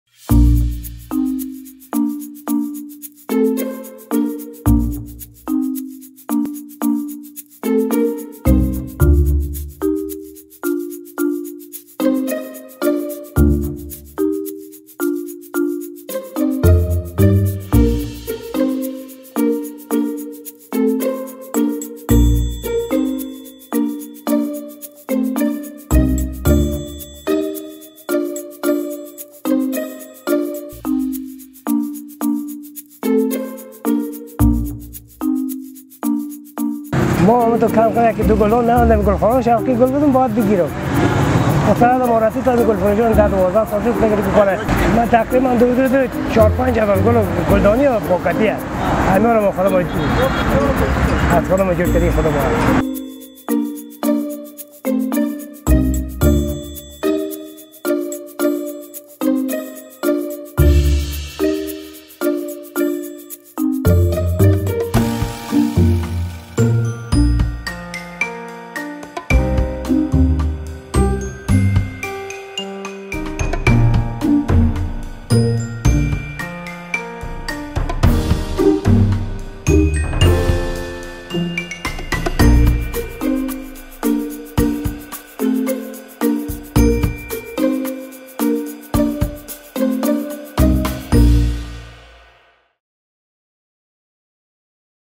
لیلی و مجنون، محبوب.ترین گل افغان‌ها/Layla and Majnun, Popular flower for Afghan's
این روزها فرهنگ نگه‌داری گل تازه میان کابلی‌ها گسترش یافته است. عطر‌ گل‌ها را می‌توان از دور در خیابان‌های کابل حس کرد
Flower Beautiful fresh_flower good sale